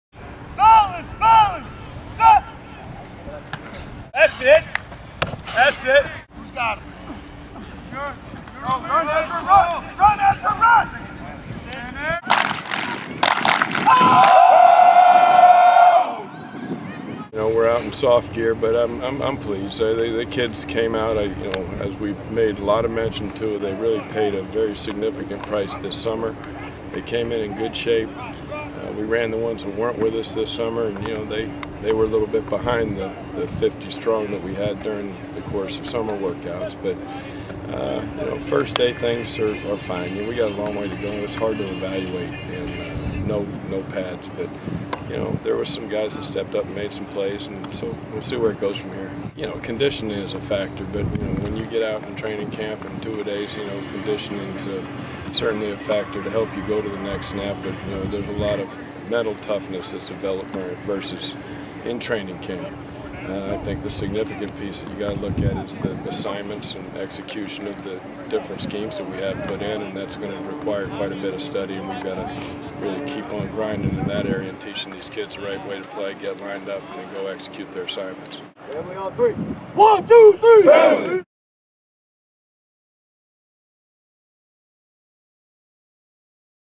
2014 Training Camp - Day 1
The 2014 season is officially underway as the Rhode Island Rams hit the practice field for the start of training camp Sunday, Aug. 10.